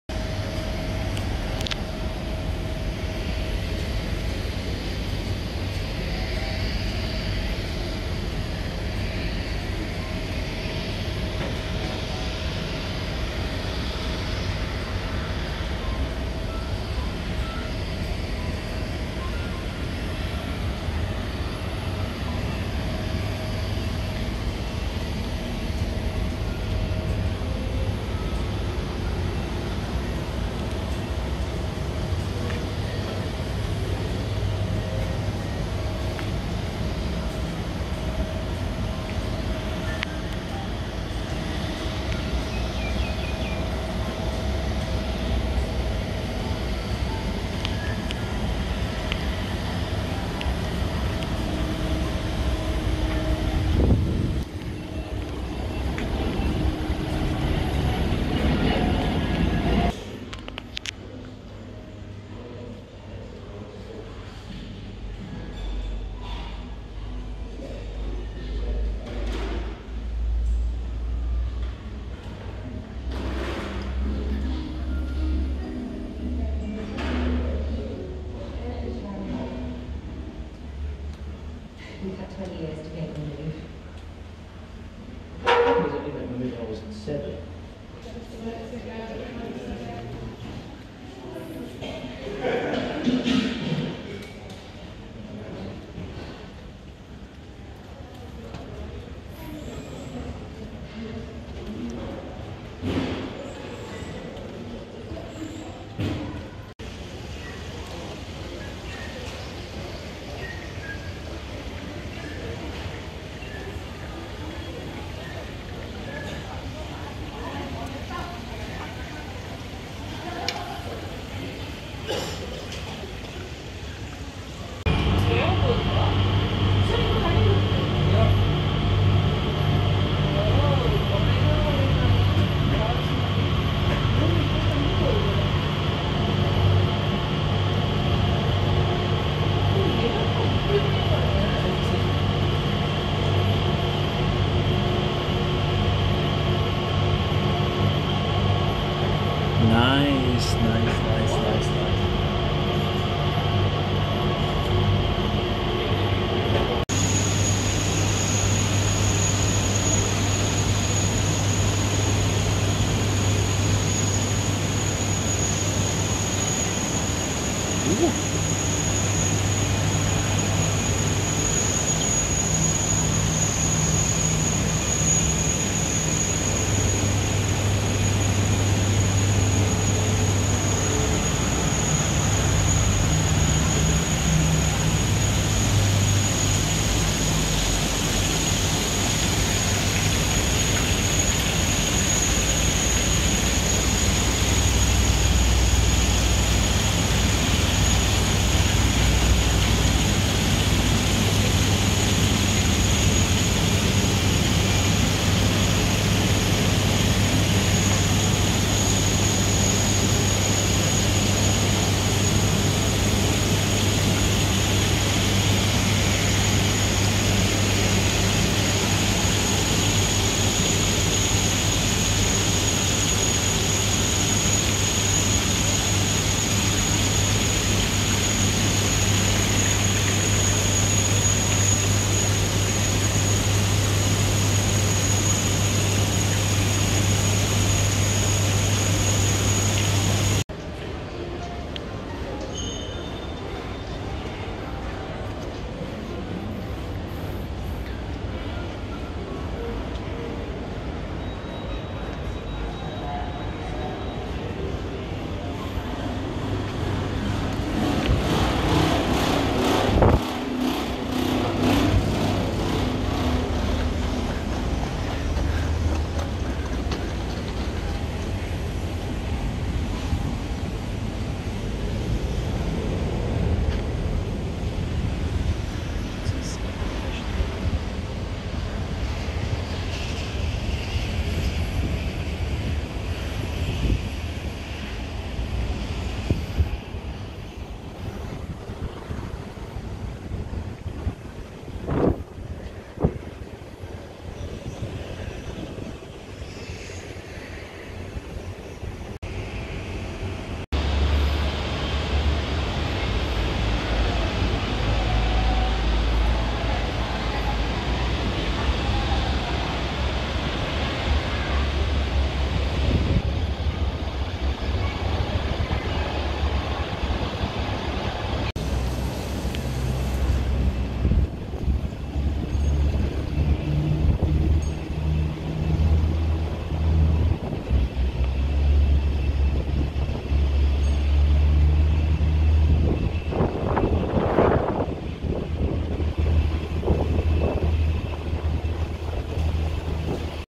The gardens @changi airport. Singapore
Explore the transit area @singapore changi airport.
- Sunflower Garden
- Cactus Garden
- Butterfly Garden
- Waterlily Garden
- Discovery Garden